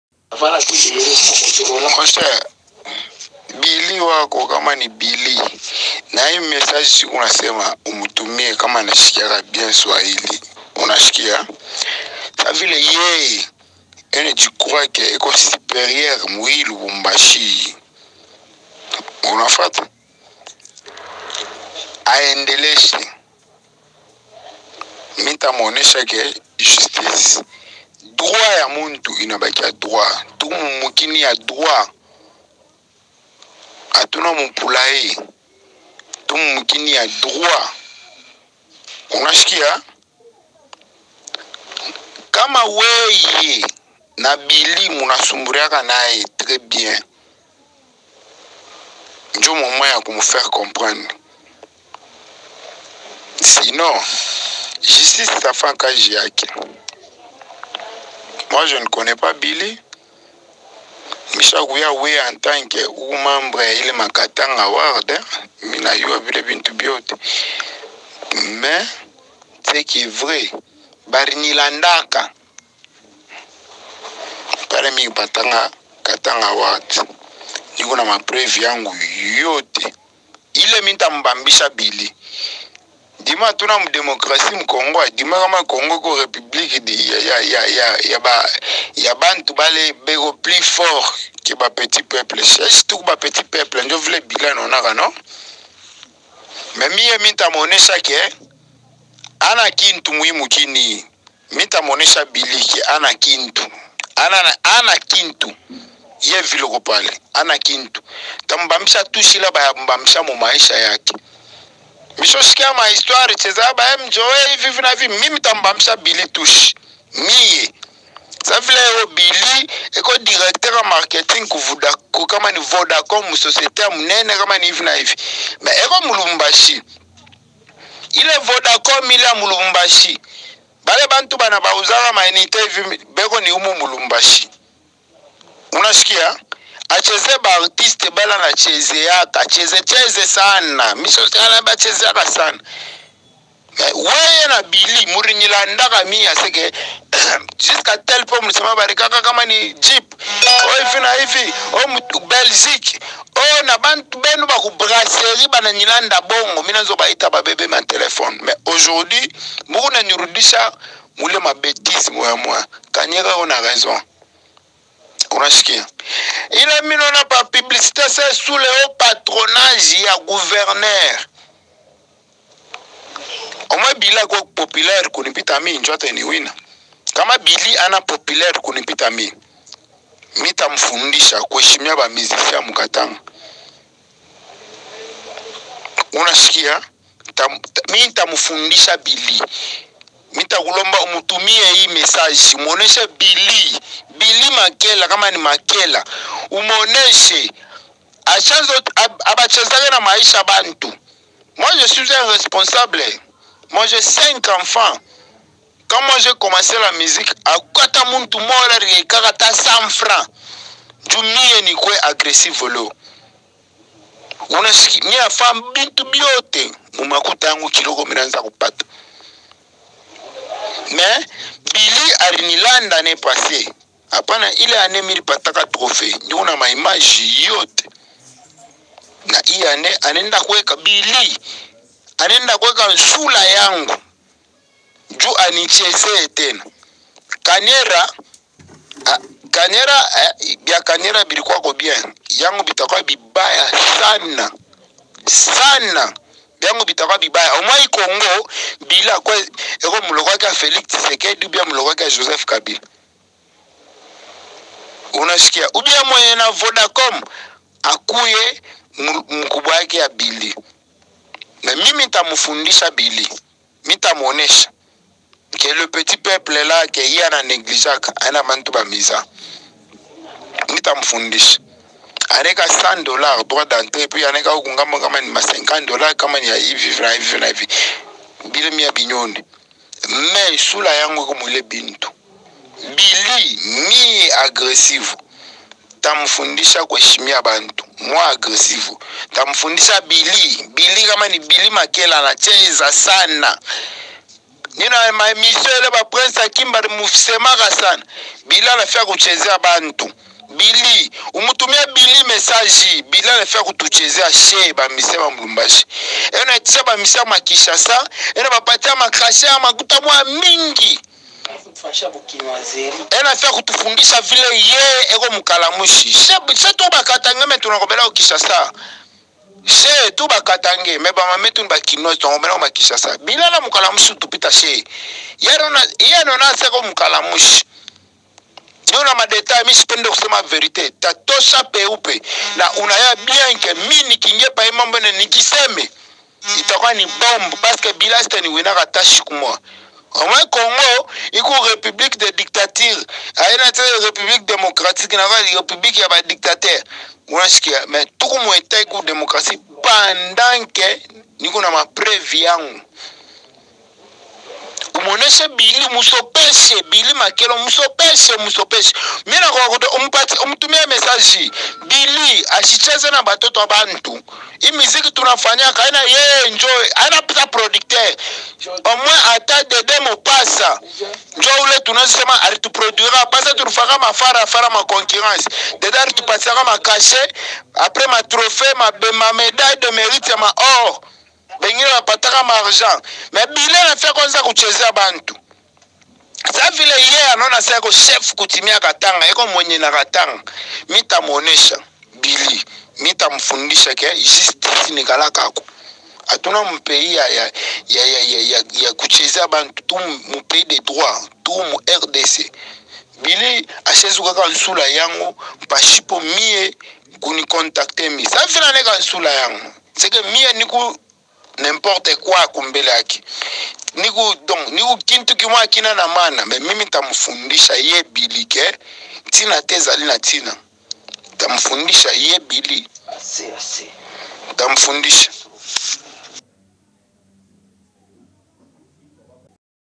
Agressivo Depose Sa plainte contre Katanga Award
Agressivo Dépose Sa plainte contre Katanga Award d'avoir Affiché Sa photo Parmi Les nominés dans la categorie Meilleur Artiste de l'année.